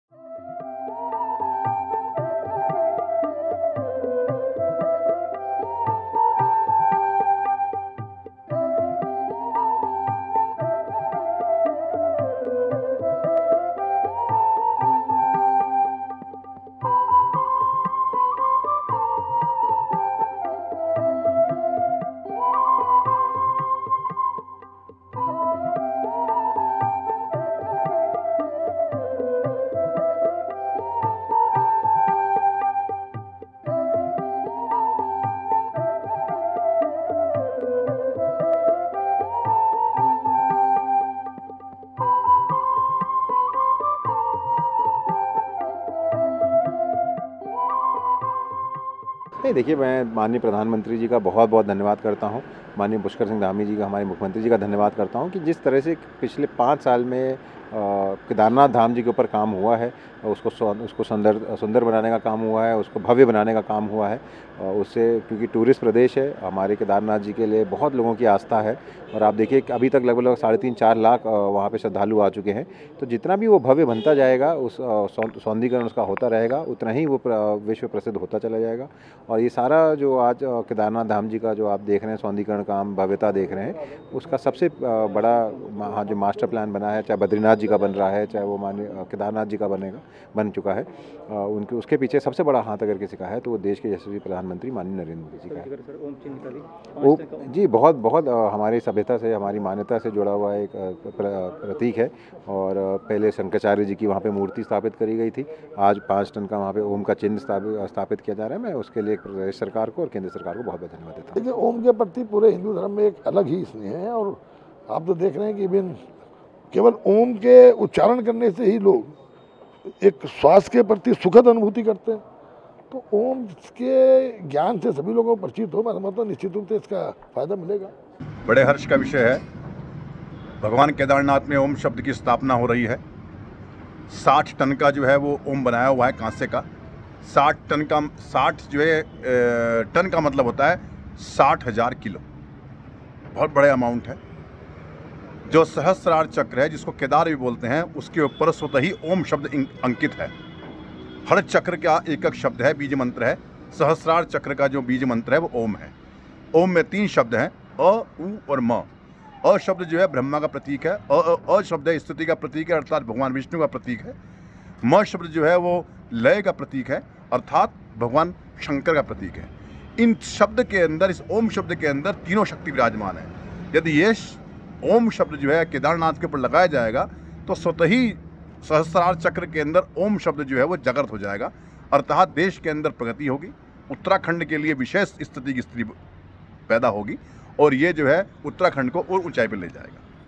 केदारनाथ में स्थापित किया जा रहा 60 क्विंटल वजनी का ॐ, ओम के बाद लगाया जाएगा विशालकाय कलश
केदारनाथ धाम 60 क्विंटल वजनी ओम की स्थापना की जा रही है. ये ओम गुजरात में बनाया गया और विशेष वाहन से केदारनाथ लाया गया. धाम परिसर में ओम के स्थापित करने का काम लगभग 80 प्रतिशत पूरा कर लिया गया है.